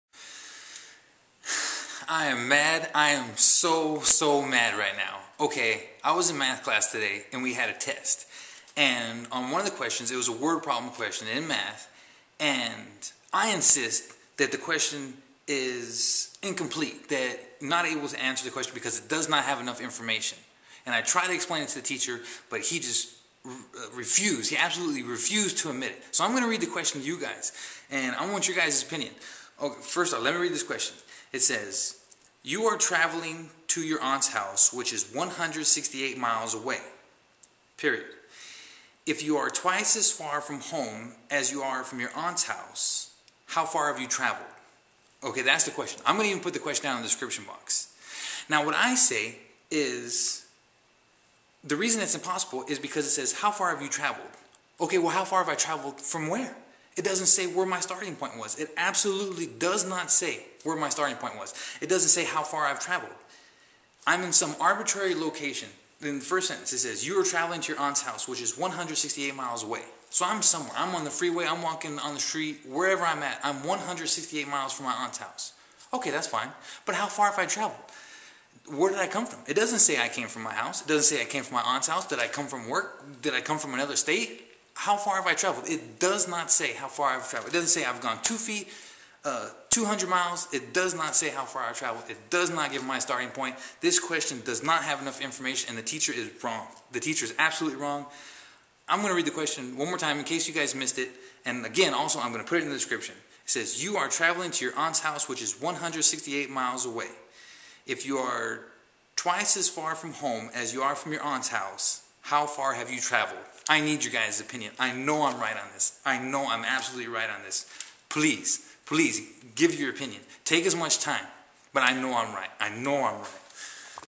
Takin it to the internet
You are traveling to your aunts house which is 168 miles away. If you are twice as far from home as you are from your aunt's house, how far have you traveled?